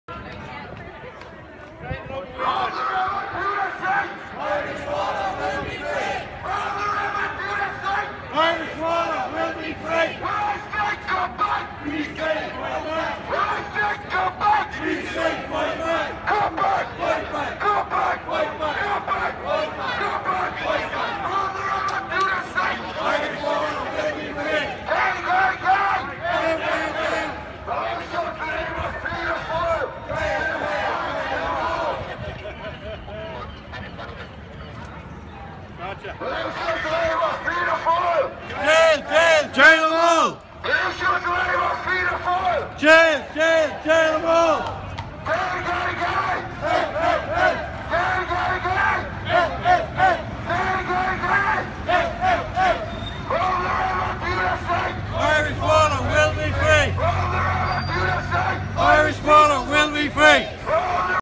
Mansion House, Dawson Street 16th November 2014.
Protest waiting for Enda "the line" Kenny at the Mansion House Sunday 16th Nov 2014.